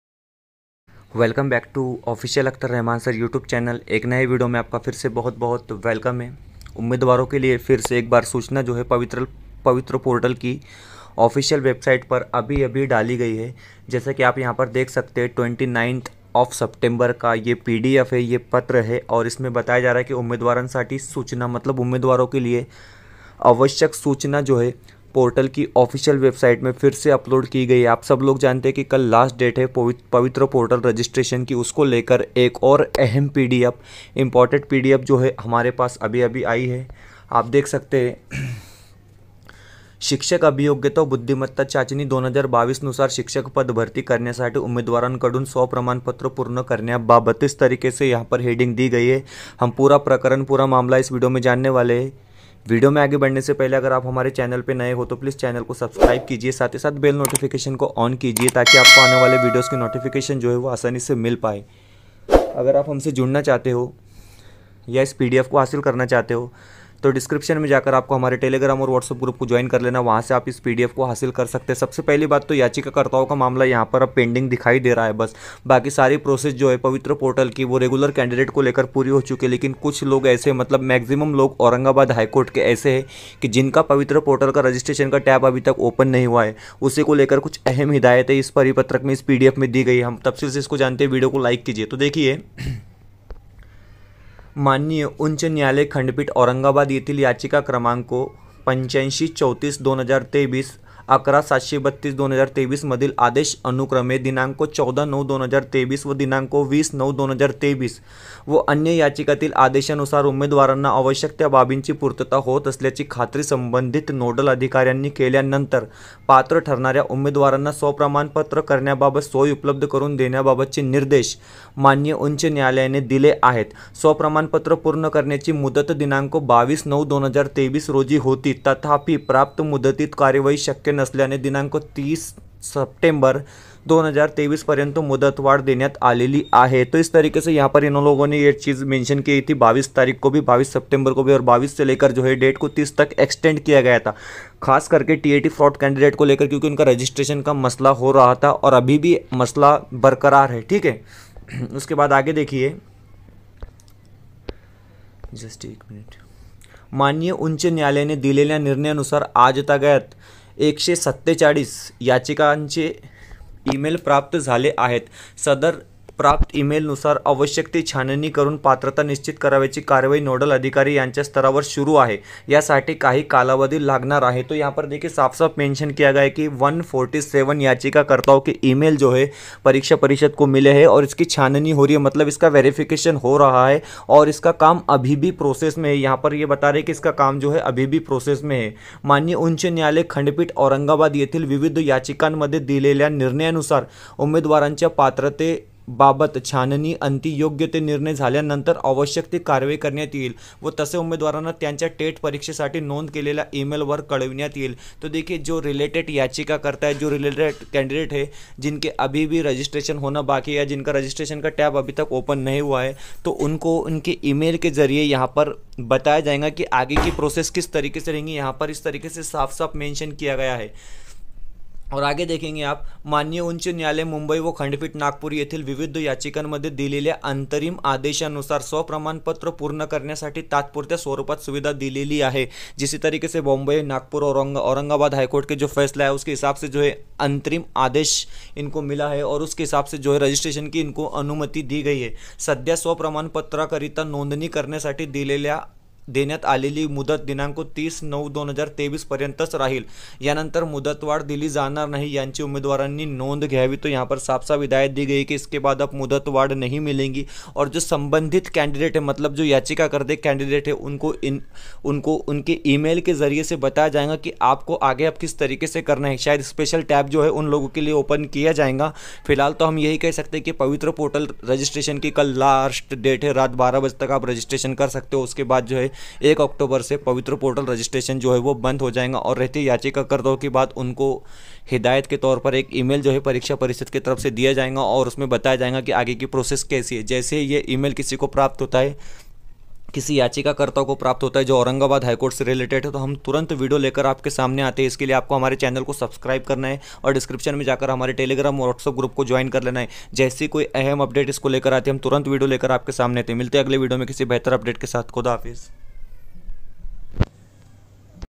पवित्र पोर्टल वर उमेदवारांसाठी पुन्हा नवीन सूचना. मुदतवाढ दिली जाणार नाही असे नवीन पत्र.
शैक्षणिक माहिती साठी आमच्या Telegram  channel किंवा WhatsApp group join करा.. 👇🏻👇🏻👇🏻

Akhtar Raheman
kanhale bk tel bhusawal
Jalgoan Maharashtra